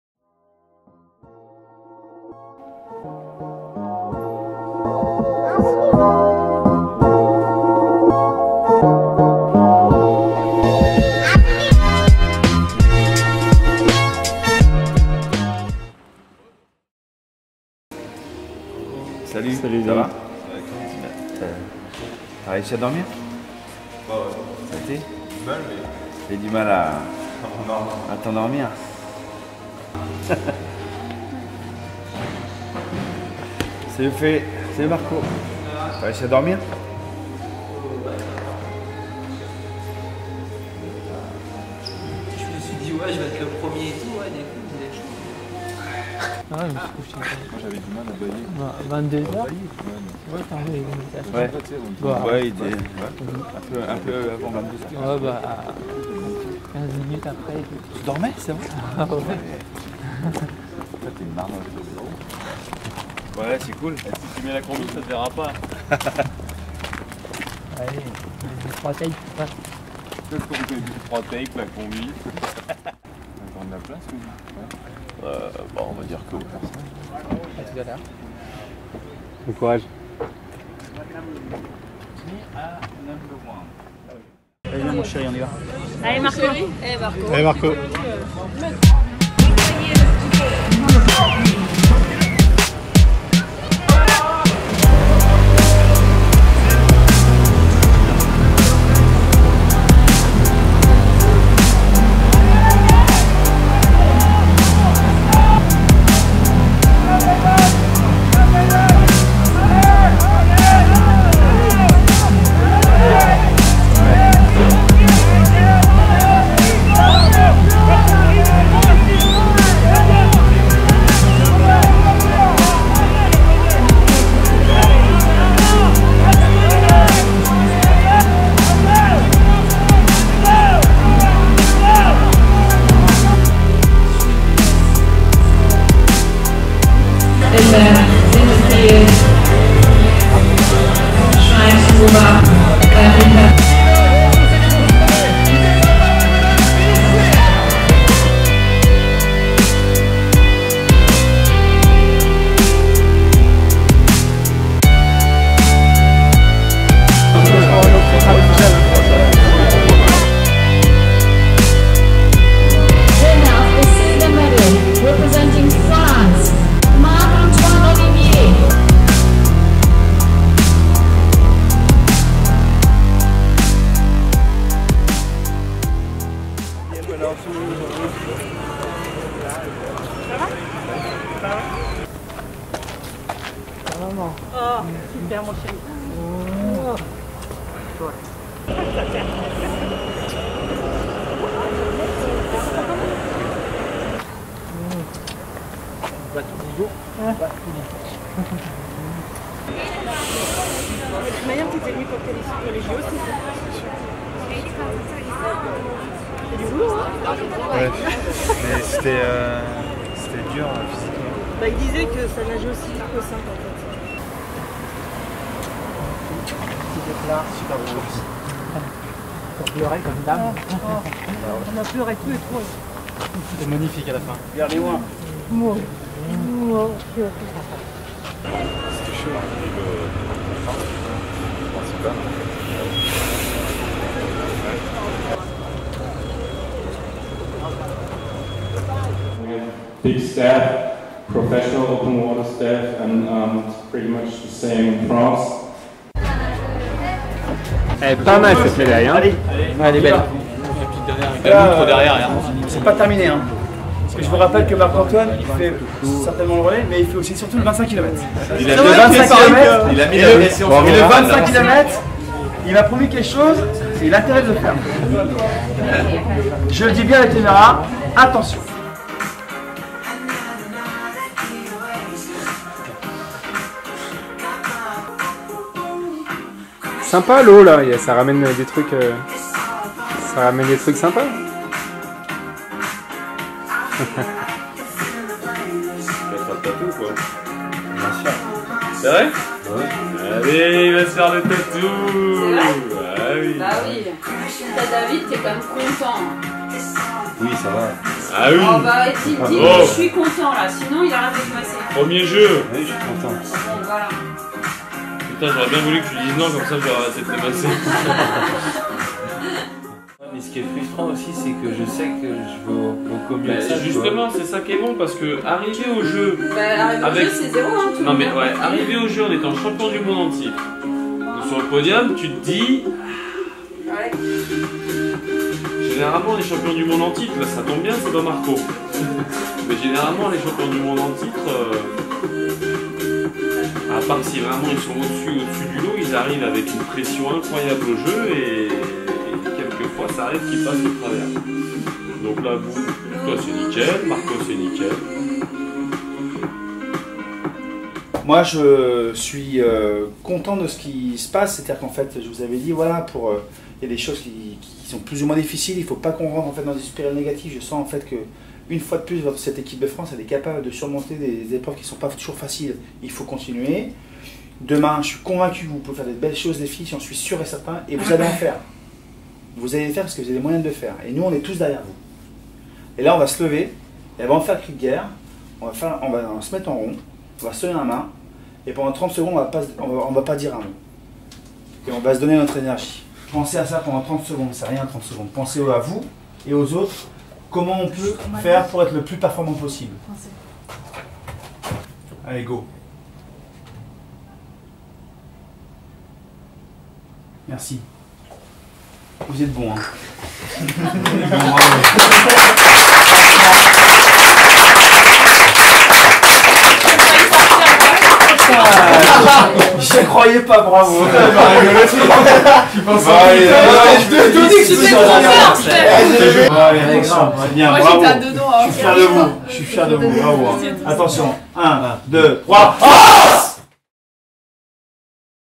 Au coeur de la qualification olympique de Marc-Antoine Olivier et David Aubry
Découvrez le cinquième épisode d'Instant Bleu, la série qui suit l'équipe de France d'eau libre durant les championnats du monde de Yeosu !